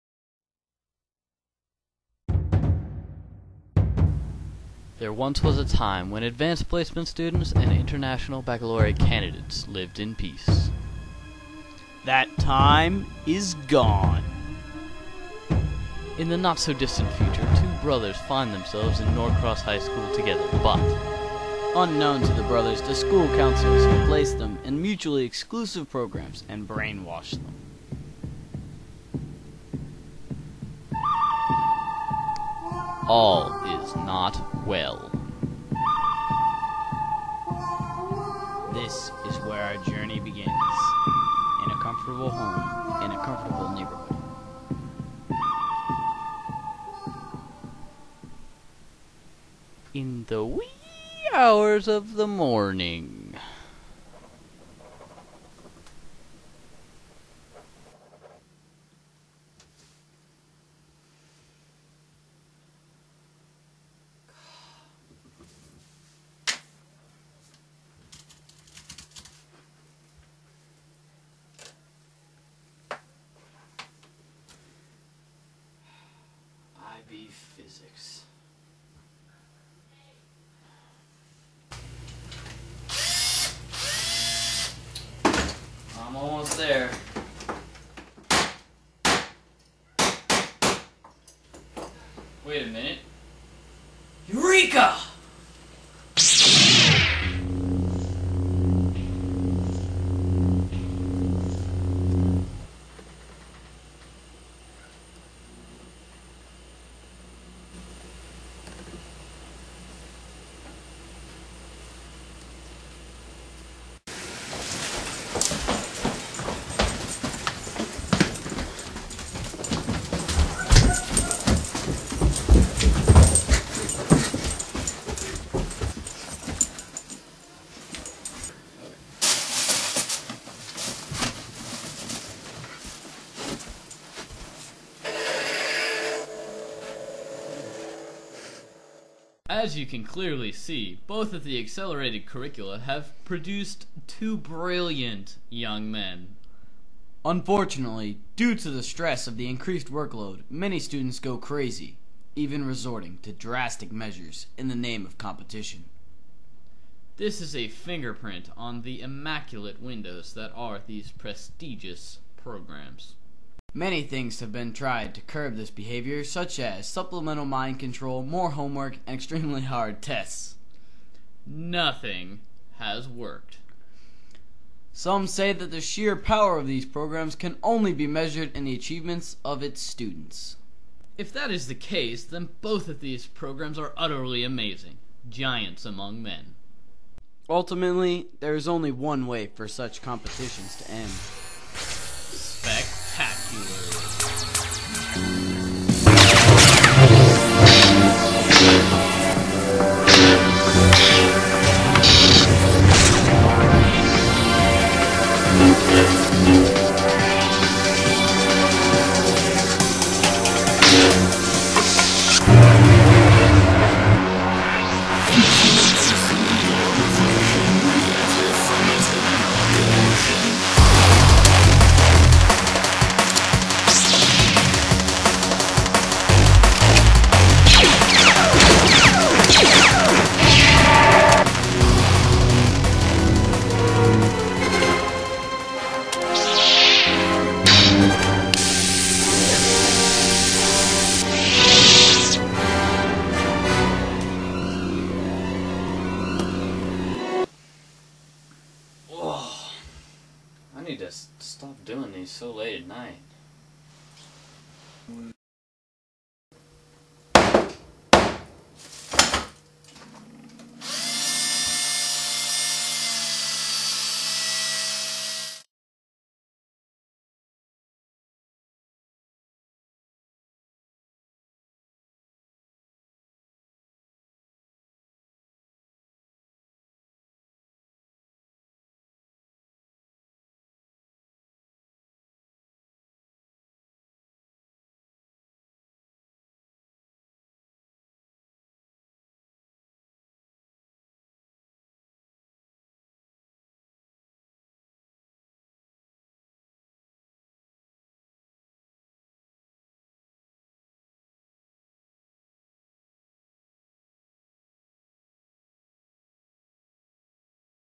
Media Festival '08
What happens when students from AP and IB collide??  Find out in this movie of epic proportions!